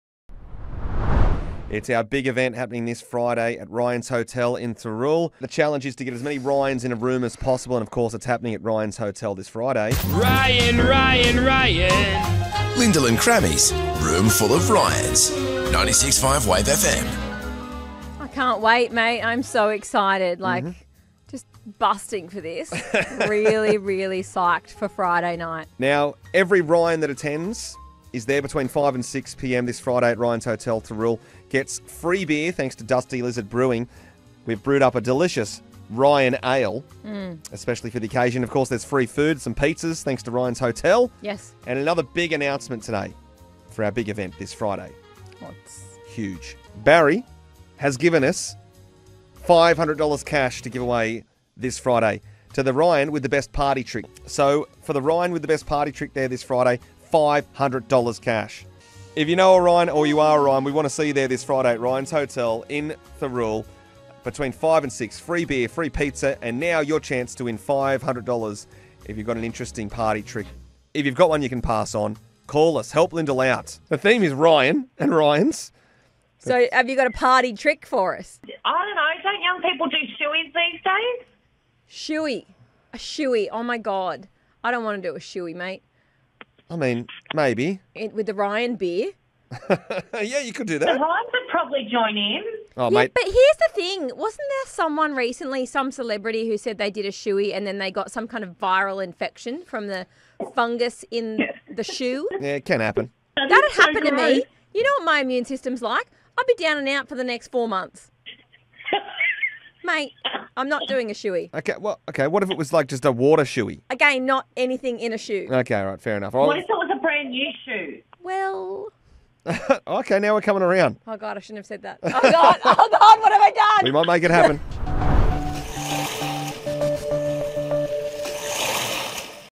What Has Lyndal Signed Up For
A Room Full of Ryans is just around the corner and Lyndal is doing well to disguise her excitement.
Ryan's Hotel and you could Win yourself $500!! + Free Beer & Pizza. 🍺 🍕
Friday 21st July, 5 - 6 pm. Ryan's Hotel Thirroul. Free Beer & Food for all Ryans. Lyndal & Crammy.